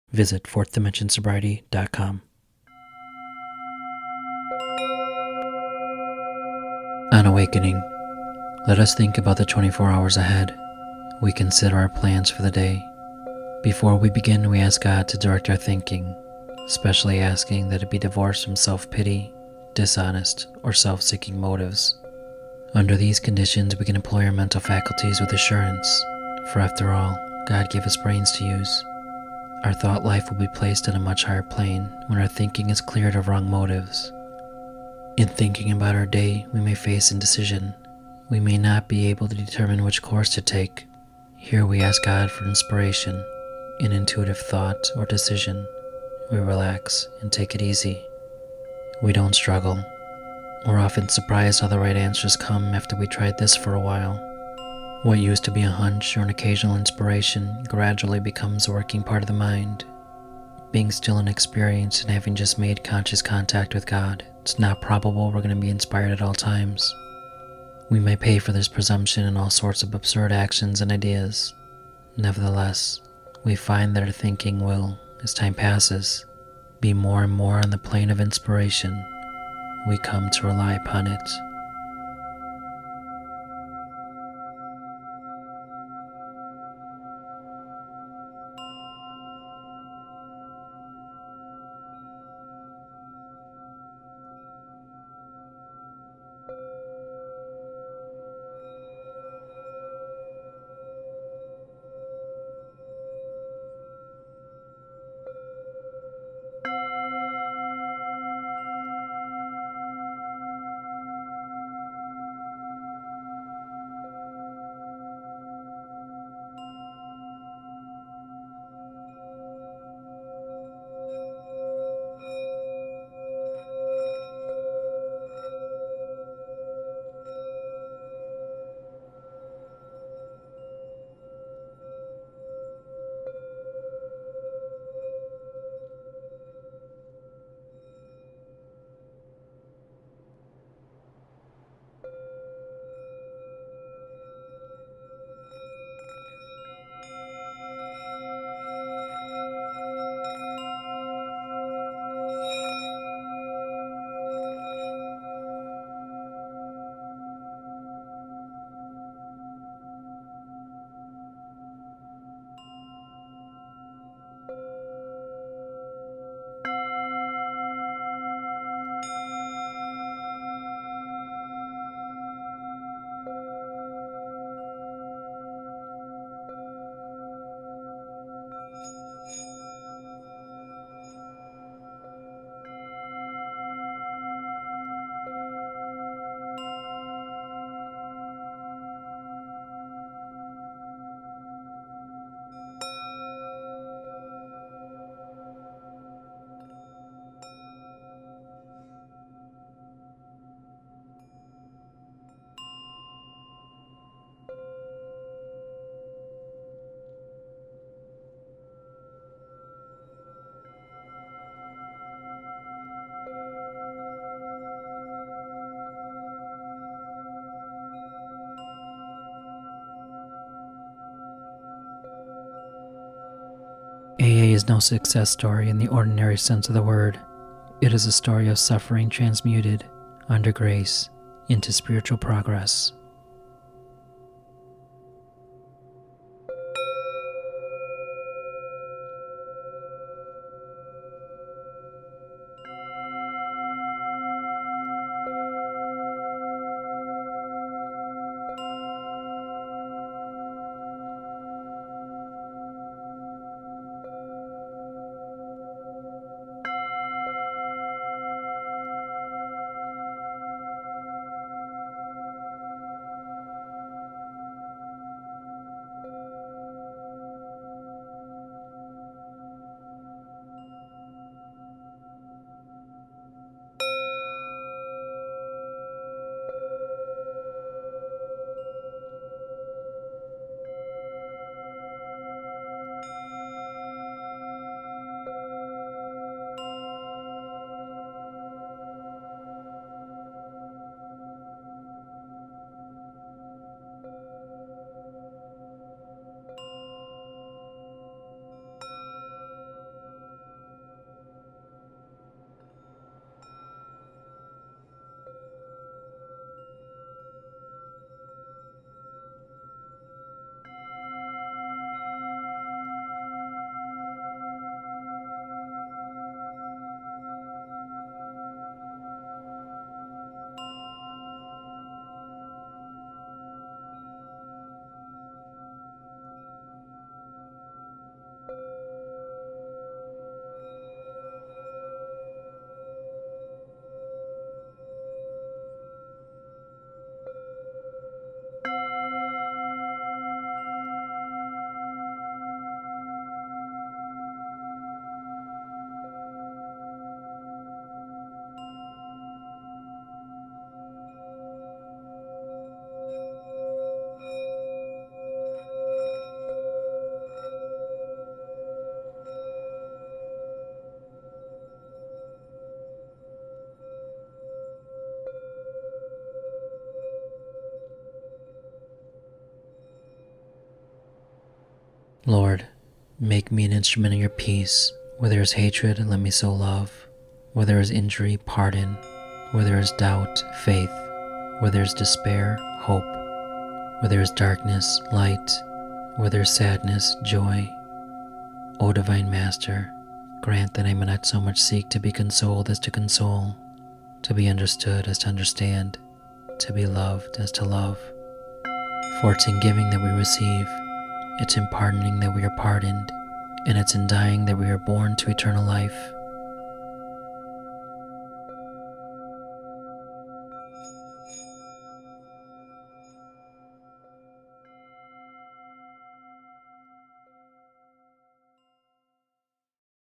11th Step: C Chakra / Suffering Transmuted
TO HELP: 4th Dimension Sobriety is a 501c3 nonprofit. 100% of donations go directly towards people's stay at 4th Dimension

THE WHY:
Every morning, at 4th Dimension Sobriety, we ask community members to participate in a non-denominational, non-religious, practice of group mediation and contemplation. Our mediation is based on the Upon Awakening, which is found on page 86 of the book Alcoholic Anonymous and conclude with 11th step prayer (The Saint Francis Prayer).

THE PROCESS:
1) On Awakening...followed by silence and reflection
2) Reflect on a reading from the Big Book of Alcoholic Anonymous...followed by silence and reflection
3) The Saint Francis Prayer

THE CHAKRA:
Root/Red/Tone C
Root Chakra, 1st Chakra
The Root Chakra is the first chakra and is located at the base of the spine. It is the root of your being and establishes the deepest connections with your physical body, your environment and with the Earth. The energy of the Root Chakra allows us to harness courage, resourcefulness and the will to live during trying times. It connects us with spiritual energies of our ancestors, their challenges and their triumphs. Since Base Chakra carries our ancestral memories, basically everyone experiences challenges or blockages within muladhara. These memories are imprinted in the subtle body and are passed down from generation to generation creating unconscious generational patterns. It is our work to take responsibility for our own lives and bring to light that which is unconscious.